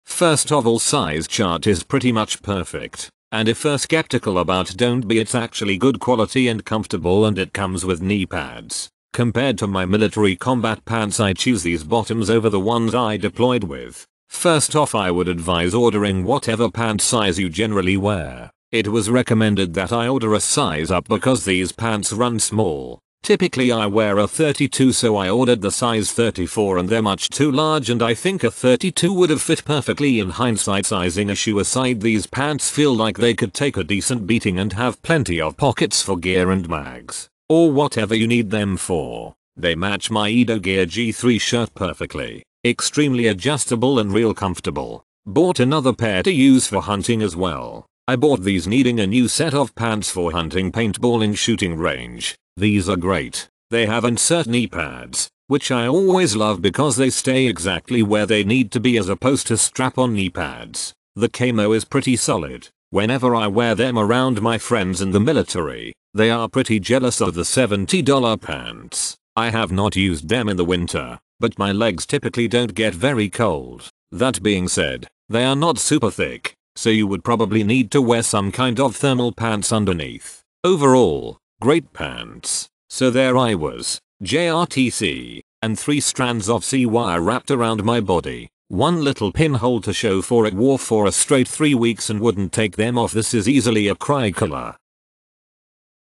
IDOGEAR G3 Combat Pants Multicam Trousers with Knee Pads for Men Tactical Pant review
IDOGEAR G3 Combat Pants Multicam Trousers with Knee Pads for Men Tactical Pant Mulitple Pocket Hook-and-Loop Adjuster

Main Features:
- MATERIAL: Made of 50% and 50% polyester with a 4-way stretch-woven accents ripstop clothing , Teflon coating, YKK zipper, military level elastic, velcro and thread, Strong stretchable clothing around knee and hip and crotch, which makes it very comfortable and durable.
- FEATURES: a) It is a NEW design IDOGEAR G3 combat pants .b) Come with the removable combat knee pads and concealed elbow pads.c) Unique low-profile waist adjust system and knee pad height adjust system makes it very professional and functional. d) Size adjustment at the back of waist, knee and ankle position e) Up to 10 different size pockets, large carrying capacity. f) Zip fly with velcro closure, 2 hip pocket with zipper closure.
- SPECIFICATION: a) It's in European regular size, a little smaller than US size, please refer to our size chart from the last photo before ordering. b) 5 sizes for your choice: Small (30W/31L), Medium (32W/32L), Large (34W/32L), X-Large (36W/33L), XX-Large(38W/33L). c) Color: MultiCam, MultiCam Black, Ranger Green.
- USAGE: Perfect for airsoft, paintball, hunting, shooting, wargame, daily wear, training ...
- PACKAGE INCLUDE: pants x 1 ( the boots and belt are not included)